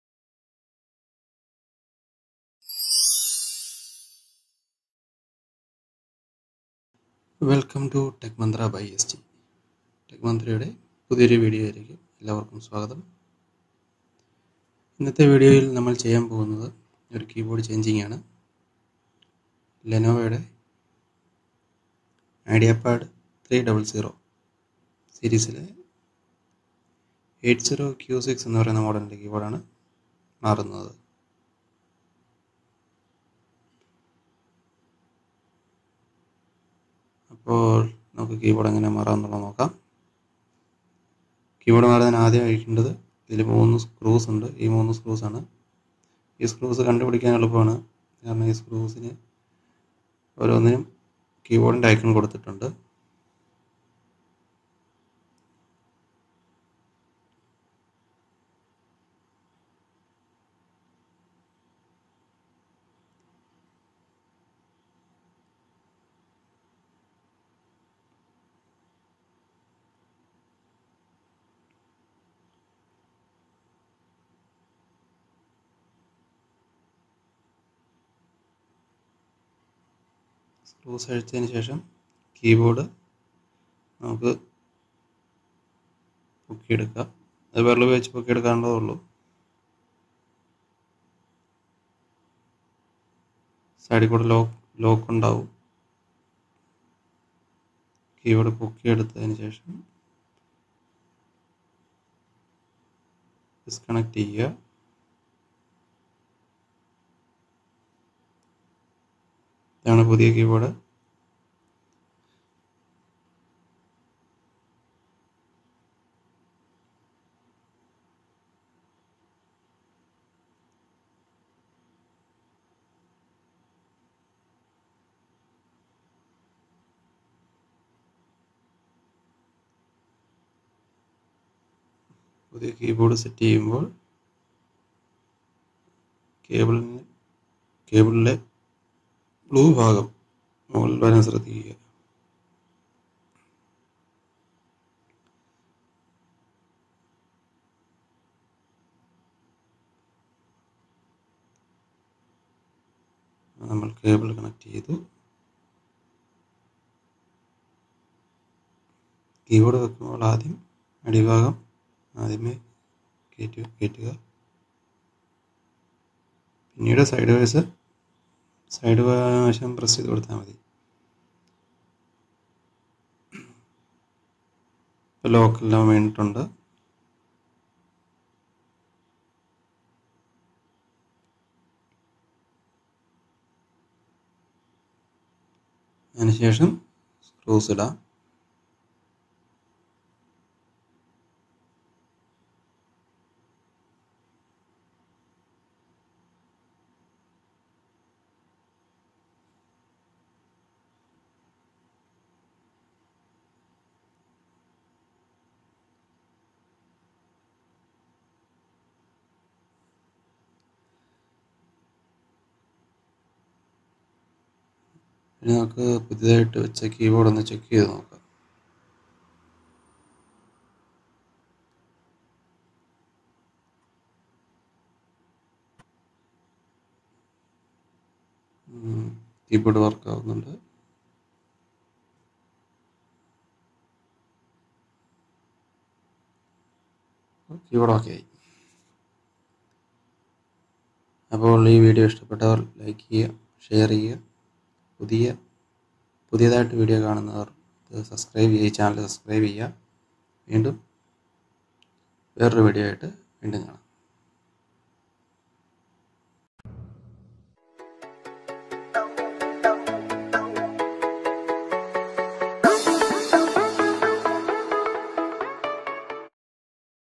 Lenovo IdeaPad 300 80Q6 Keyboard Remove | Replacement DIY
Lenovo Ideapad Remove,Replace Keyboard DIY
How to remove replace lenovo ideapad keyboard
Lenovo Ideapad 300-14ISK
Model : 80Q6